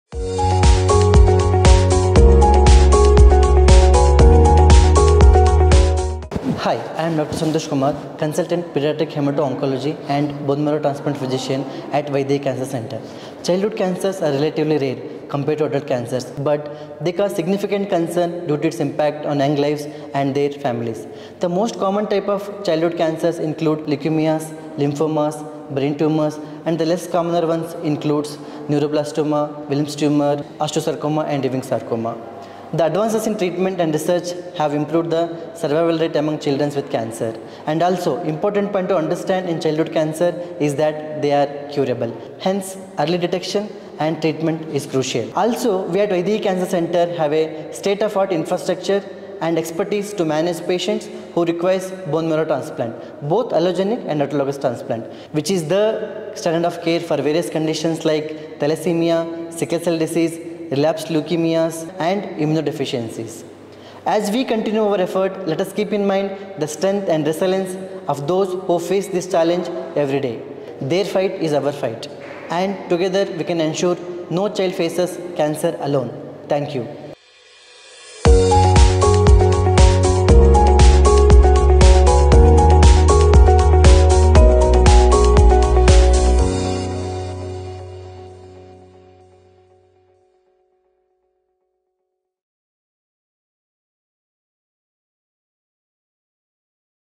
Dr. Santosh’s Take on Pediatric Cancers |Tech Driven Advanced Care | Vydehi Cancer Centre
Join Dr. Santosh Kumar, a leading expert in pediatric oncology, as he discusses the latest advancements in treating childhood cancers. Learn about the most common types, early detection, and the vital role of bone marrow transplants. Discover how Vydehi Cancer Center is at the forefront of providing compassionate and effective care for young patients.